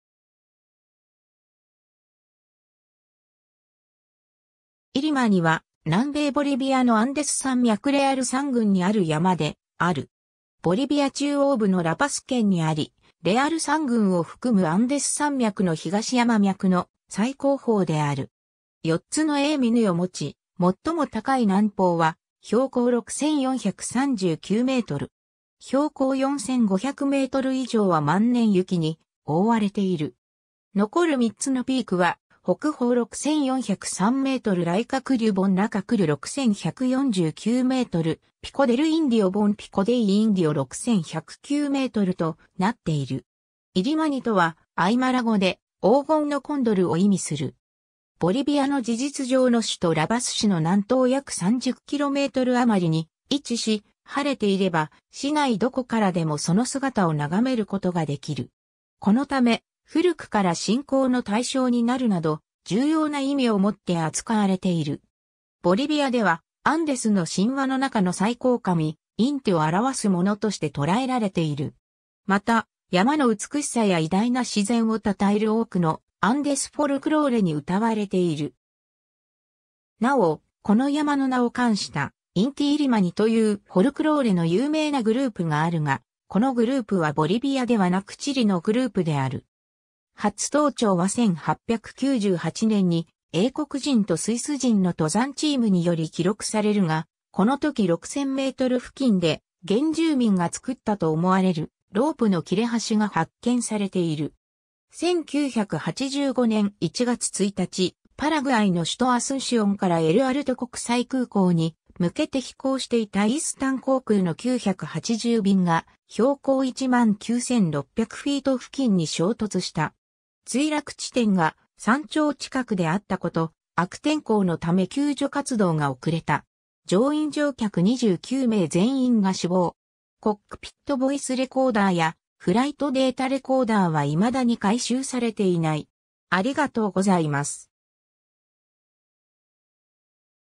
イリマニ
ラパス県_(ボリビア)
イリマニ（IllimaniまたはNevado Del Illimani）は南米ボリビアのアンデス山脈レアル山群（Cordillera Real）にある山である。
 ボリビア中央部のラパス県にあり、レアル山群を含むアンデス山脈の東山脈（Cordillera Oriental）の最高峰である。
4つの鋭峰を持ち、最も高い南峰は標高6,439m。
標高4,500m以上は万年雪に覆われている。
残る3つのピークは、北峰（デ・パリス峰De Paris）6,403m・ライカ・クリュ峰Layca Kullu6,149m・ピコ・デル・インディオ峰Pico Dei Indio6,109mとなっている。
 「イリマニ」とは、アイマラ語で「黄金のコンドル」を意味する。
ボリビアの事実上の首都ラパス市の南東約30kmあまりに位置し、晴れていれば市内どこからでもその姿を眺めることができる。
このため、古くから信仰の対象になるなど、重要な意味を持って扱われている。
ボリビアではアンデスの神話の中の最高神インティを表すものとしてとらえられている。
また、山の美しさや偉大な自然を讃える多くのアンデスフォルクローレに歌われている。
 なお、この山の名を冠した「インティ・イリマニ」（Inti-Illimani）というフォルクローレの有名なグループがあるが、このグループはボリビアではなくチリのグループである。
 「初登頂」は1898年に英国人とスイス人の登山チームにより記録されるが、このとき6000m付近で、原住民が作ったと思われるロープの切れ端が発見されている。
 1985年1月1日、パラグアイの首都アスンシオンからエル・アルト国際空港に向けて飛行していたイースタン航空の980便が、標高19,600フィート（約5,970m）付近に衝突した（イースタン航空980便墜落事故）。
墜落地点が山頂近くであったこと、悪天候のため救助活動が遅れた。
乗員乗客29名全員が死亡。
コックピットボイスレコーダーやフライトデータレコーダーは未だに回収されていない。